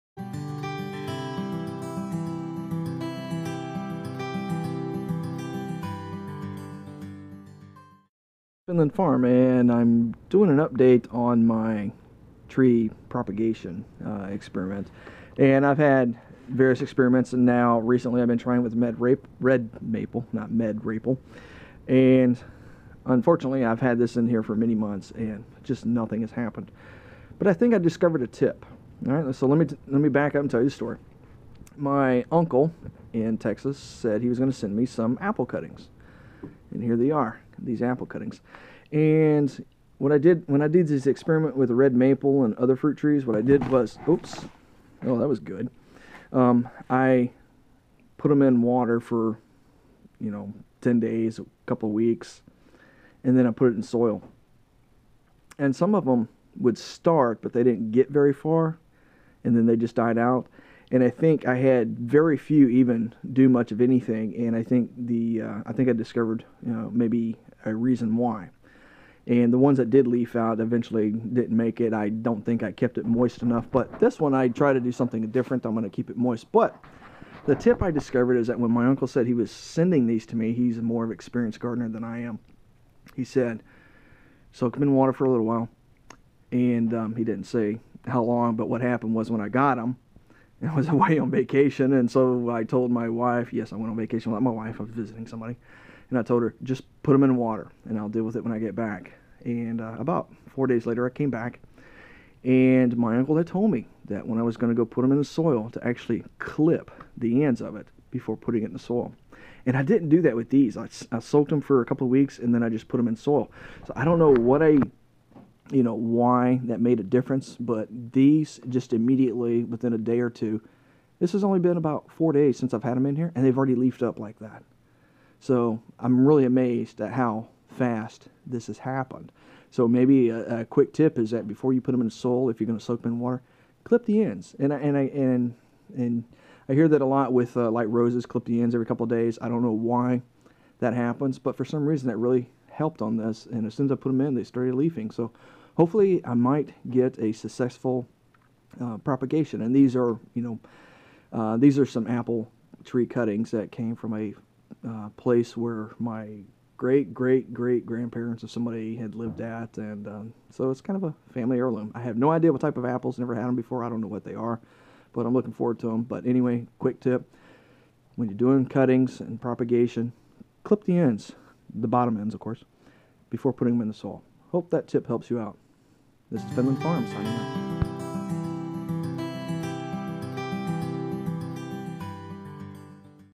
Propagating Trees with Cuttings - An important tip
Free book

If you have been having a hard time propagating trees with cuttings after soaking them in water for a while, this little tip might be of help.

PS... pay no attention to that piece of fuzz that got caught in my fingernail.